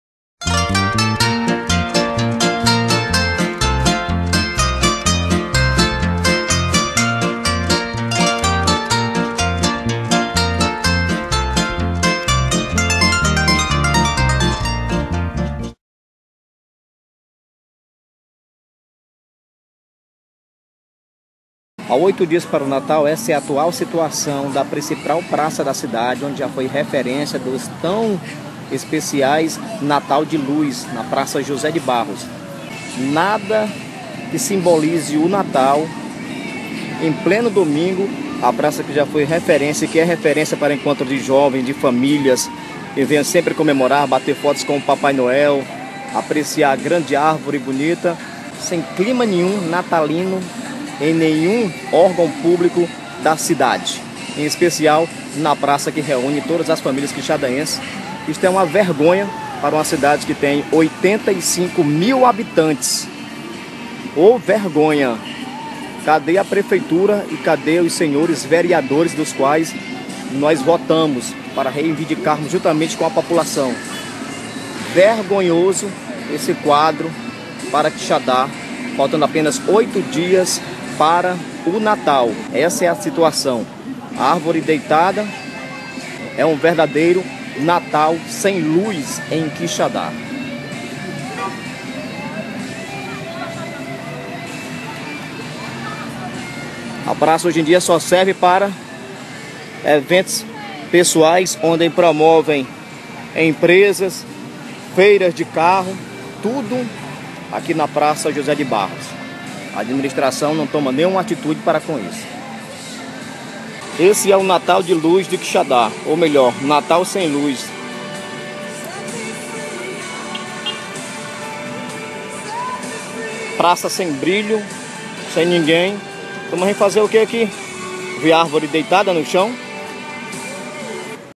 NATAL SEM LUZ
CASO ALGUEM SE SINTA OFENDIDO ACEITE MINHAS DESCULPAS
FIQUEI MUITO CHATEADO EM IR TOMAR UM SORVETE MAS A FAMILIA EM UMA SORVETERIA EM FRENTE A PRAÇA E ME DEPARAR COM UM VISÃO HORRIVEL EM Q ELA SE ENCONTRA EM PLENO MÊS NATALINO, FIQUEI OBSERVANDO E PASSOU UM FILME DEM MINHA MEMORIA DE COMO ELA LINDA A PRAÇA NESSE MES ONDA FICAMOS BATENDO FOTOS PARA ENVIAR A FAMILIARES DOM TODO ORGULHO DE COMO ESTAVA LINDA NOSSA PRAÇA. A ESPERA DELES E MUITO OUTROS TURISTAS PRA MIM ERA UM ORGULHO! E VC COMO SE SENTE COM ESSE DESCASO?